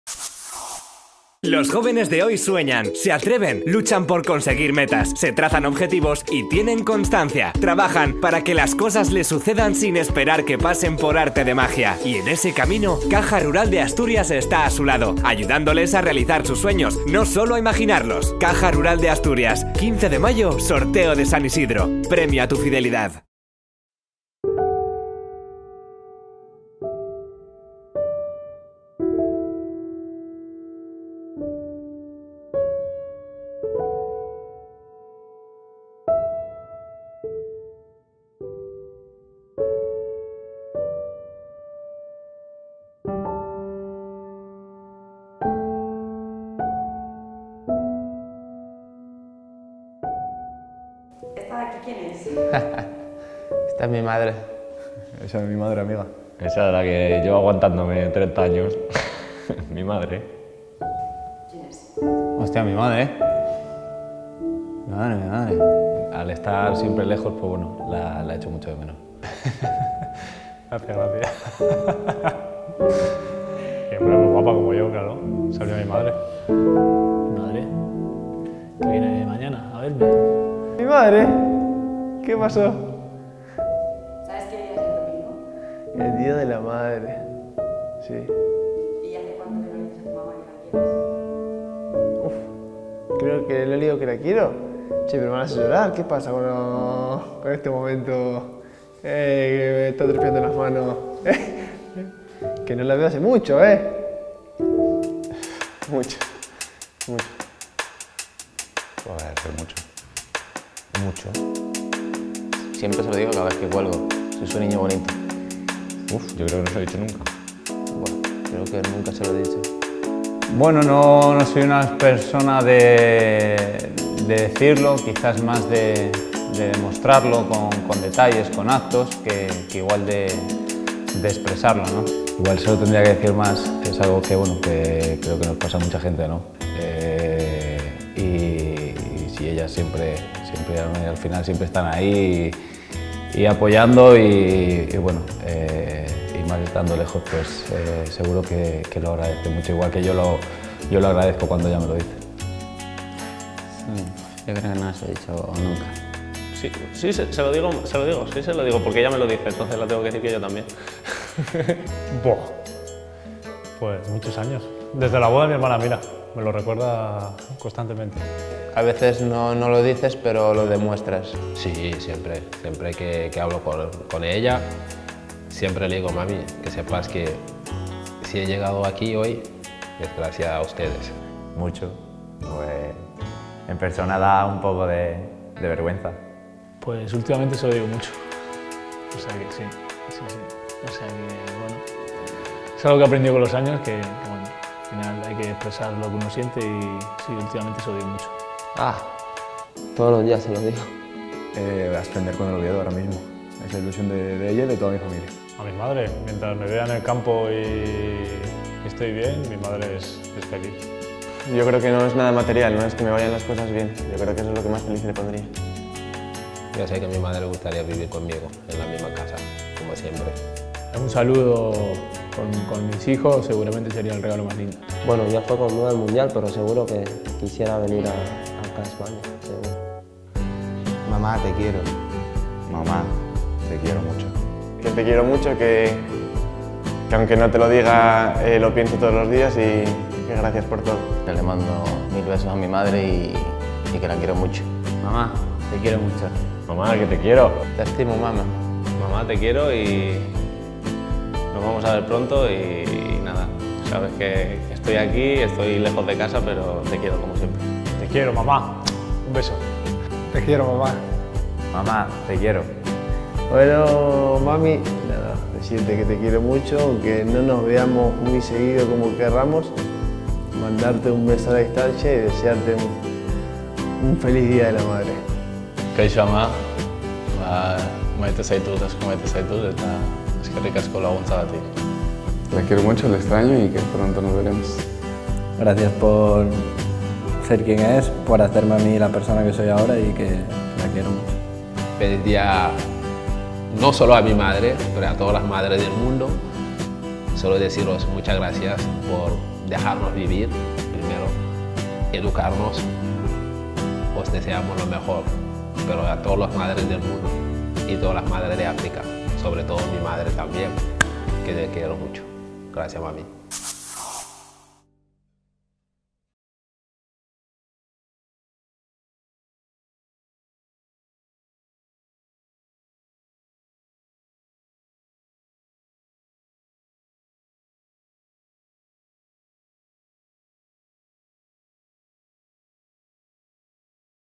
Mamá: te quiero
Les mostramos a nuestros jugadores las fotos de sus madres y les hacemos dos preguntas: ¿Cuánto hace que no le dices te quiero? ¿Cual sería para ella el mejor regalo del mundo? Este es el resultado.